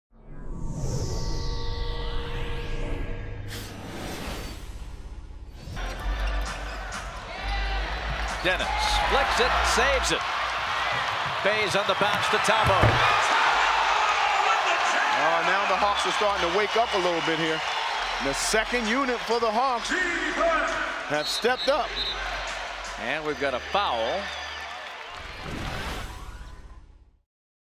Fast Break of the Night- December 26
Dennis Schroder saves the ball from going out of bounds and Kent Bazemore finds Thabo Sefolosha on the break for the slam.